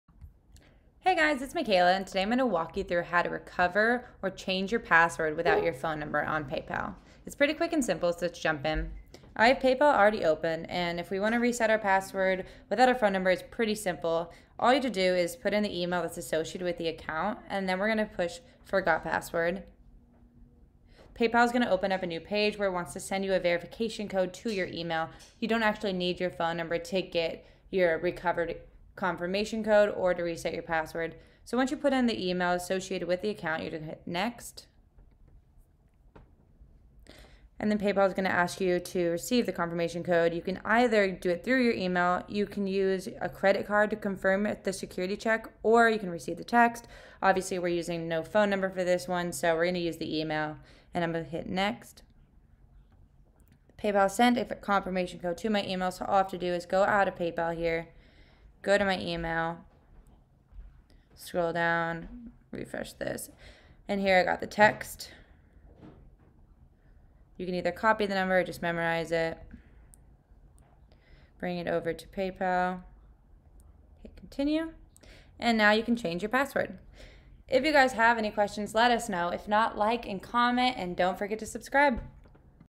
How To Recover/Change PayPal Password Without Phone Number
Learn how to recover and change your PayPal password without access to your phone number in this quick and easy guide.

PayPal gives you several different options when it comes to recovering and changing your password.

As long as you have access to your email that's associated with your PayPal account you can reset and change your password without using your phone number at all. You will need access to your email, so make sure you can login and check your email for the one-time code.